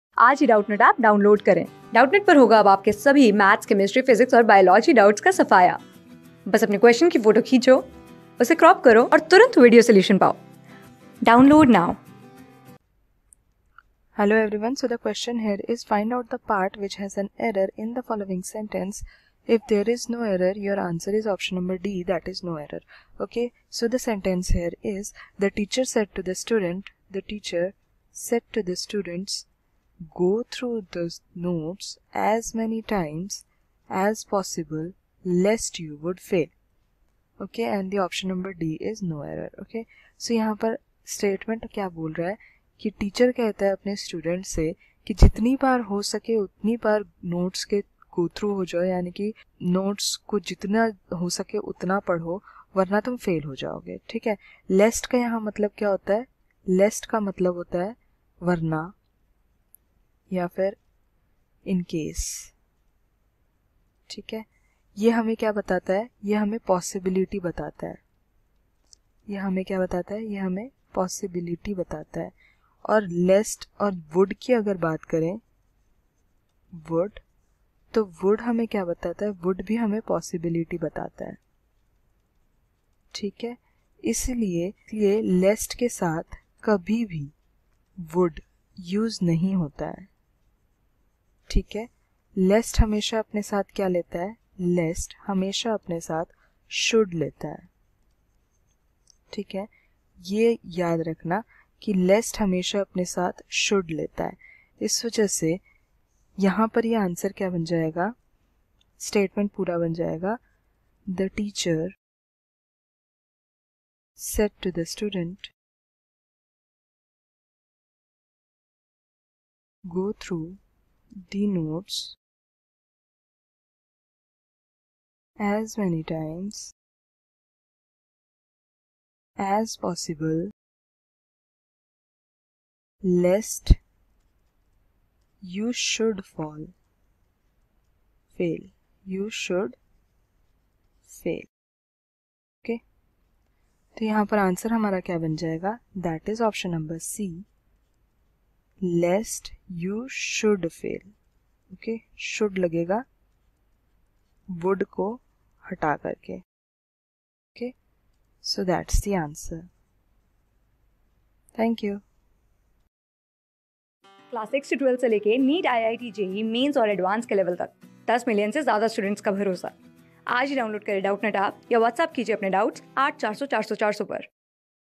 Find out the part which has an error in the following sentences. If there is no error, your ans...
Find out the part which has an error in the following sentences. If there is no error, your answer is (d).The teacher said to the students, (a)/ Go through the notes as many times (b)/ as possible lest you would fail. (c )/ No error (d)
Class: 14
Subject: ENGLISH
Chapter: COMMON ERRORS
Board:GOVT

👉 Have Any Query? Ask Us.